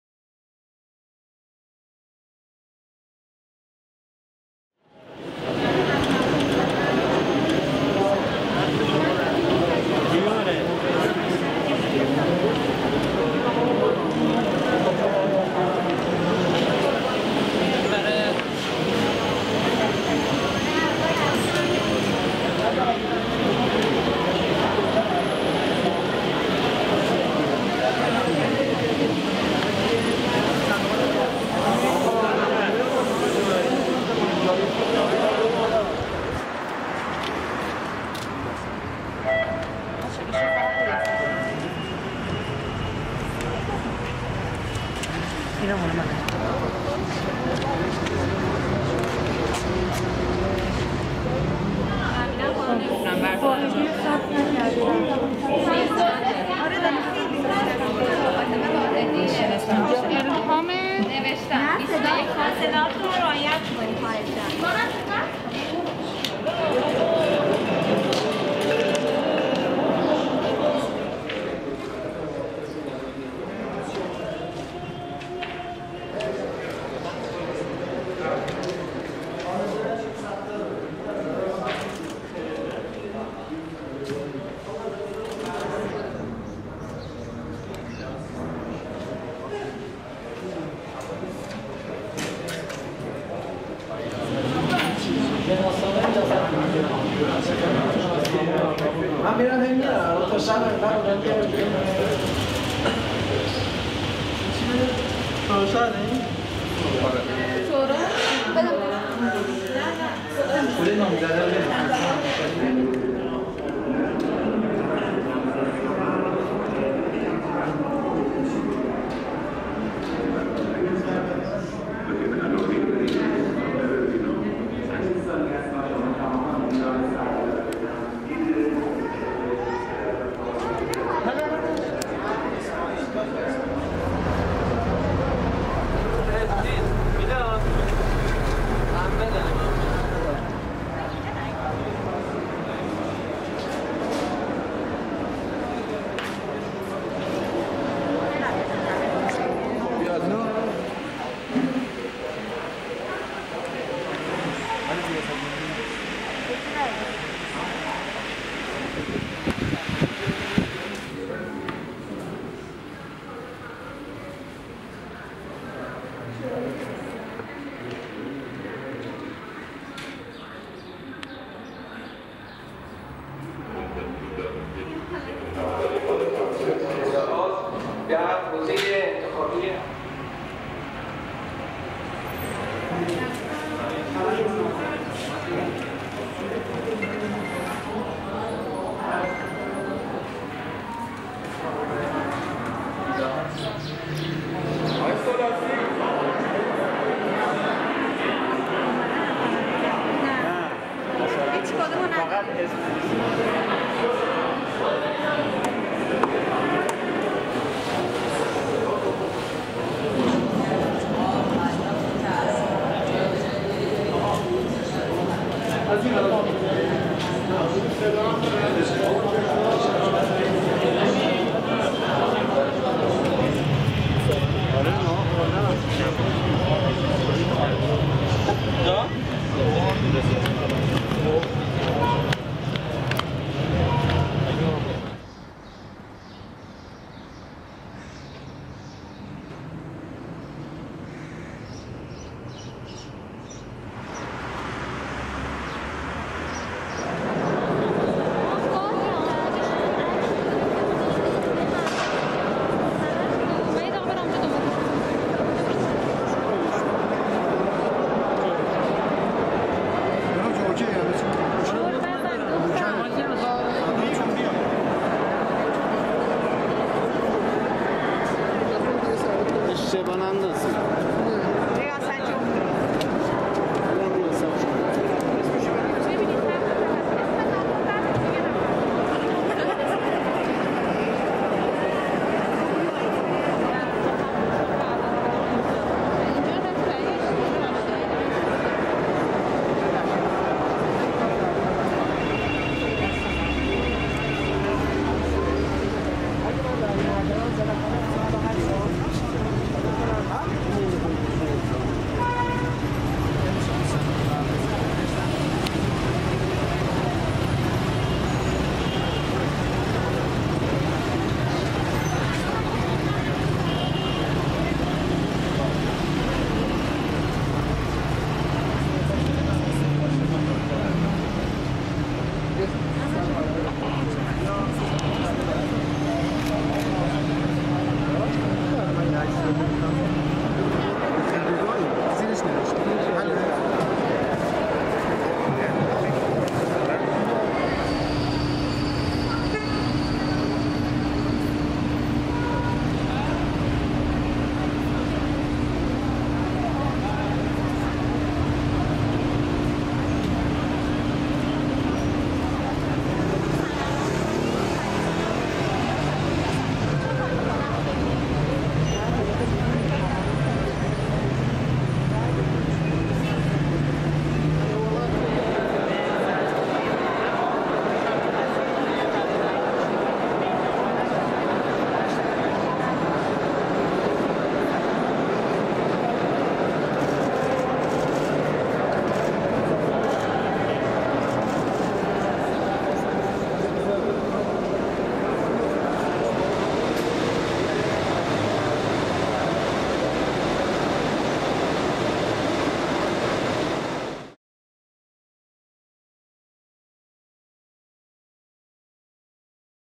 ادامه رای‌گیری انتخابات ۱۴۰۰ در تهران
انتخابات ۱۴۰۰ شامل انتخابات ریاست جمهوری و شوراهای اسلامی شهر و روستا به همراه میاندوره‌ای مجالس خبرگان و شورای اسلامی در برخی حوزه‌ها، از صبح جمعه (۲۸ خرداد) در سراسر کشور آغاز شده است. در این ویدئو گوشه‌هایی از حضور مردم در پای صندوق‌های اخذ رأی در حوزه‌های مختلف شهر تهران را در مهلت بعدازظهر امروز را مشاهده خواهید کرد.